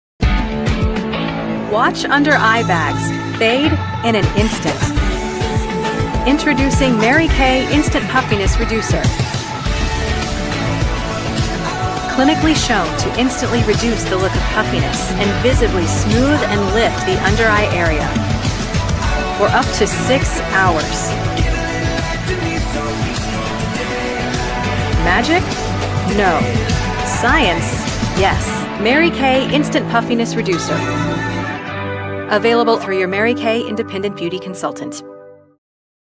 Mary Kay - New Puffy Eye Reducer
Get ready to instantly reduce under eye puffiness with our brand new, science based formula.